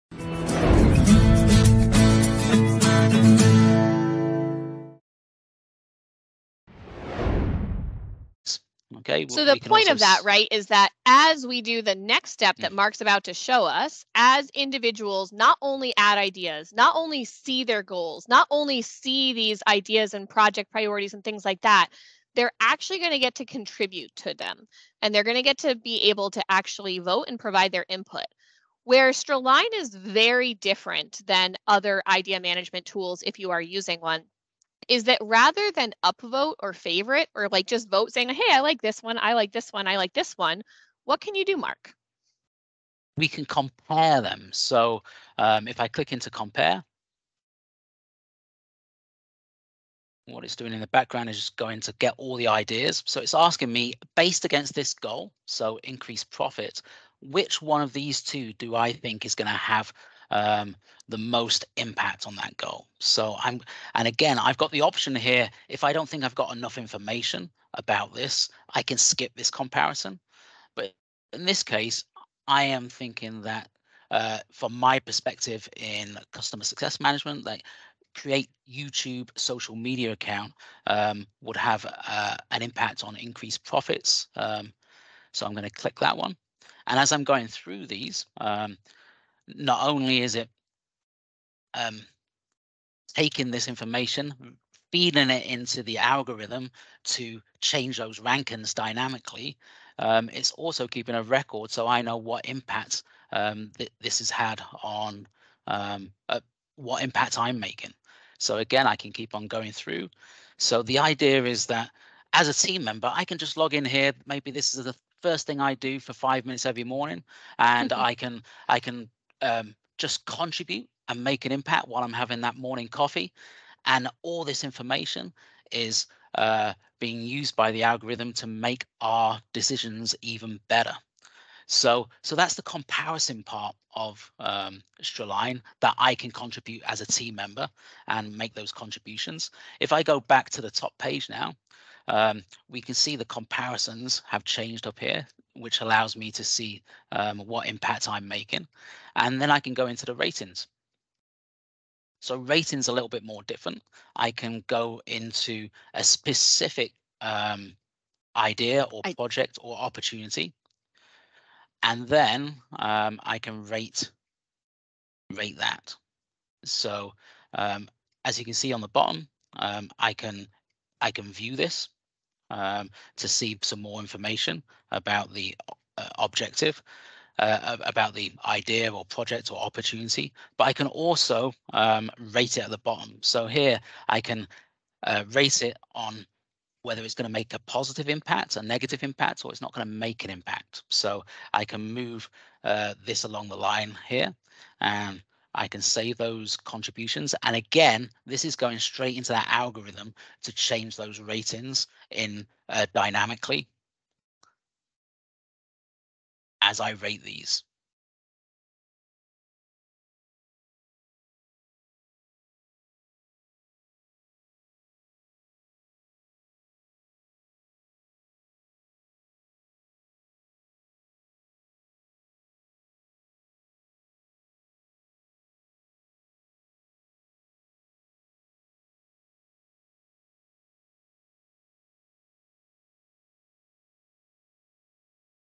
What makes StrAlign different?
Most project prioritization tools depend on upvoting and downvoting. So what makes StrAlign different?

Each user does more than just submit ideas; they can contribute to decisions by comparing ideas, and choosing which idea will have the biggest impact on strategic goals.

In this video, join Mark Monaghan and Molly Yanus as they illustrate how the compare feature of StrAlign sets it apart from other tools.

Interested to see how your team or organization could use StrAlign? Schedule a free demo of the tool with our team